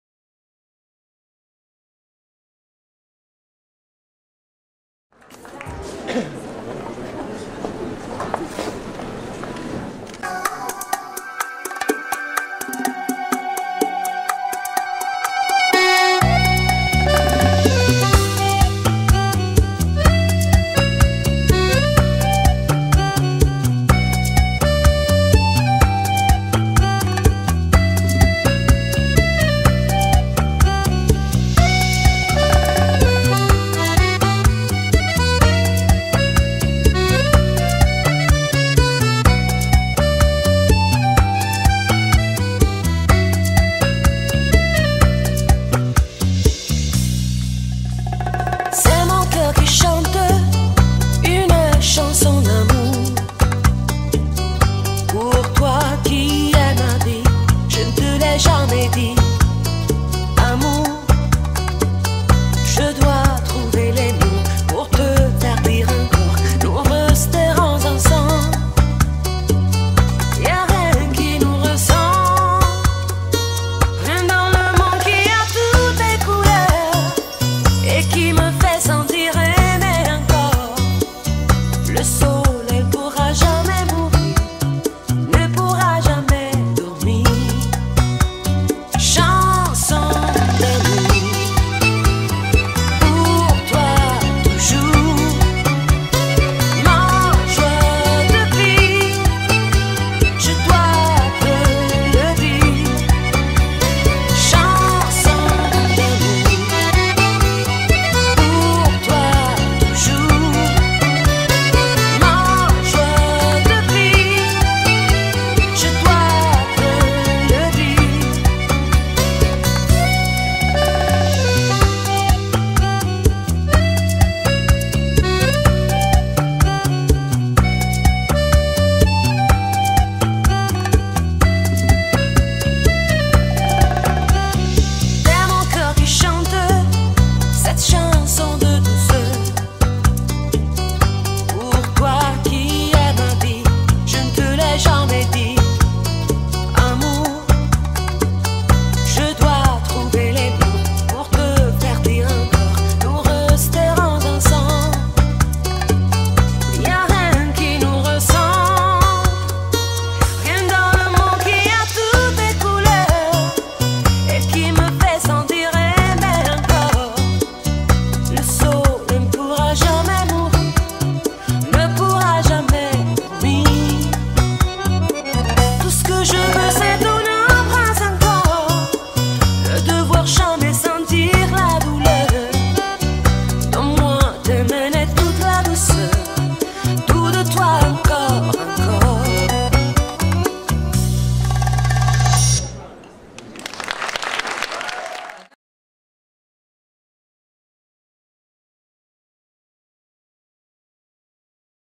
SNA 2009 Академия Танца "Бачатанго"
Выступление представителей  школы "Академия Танца" на конкурсе шоу номеров, проходившем в рамках премии Salsa Night Awards 2009/